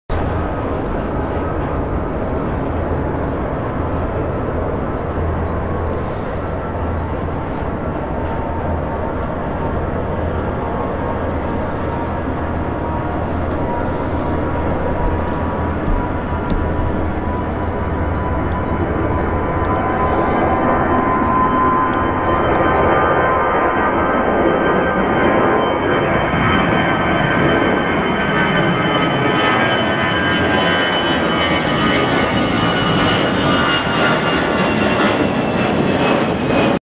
FullHD AirBerlin D-ABFG
AirBerlin D-ABFG departing at Düsseldorf International.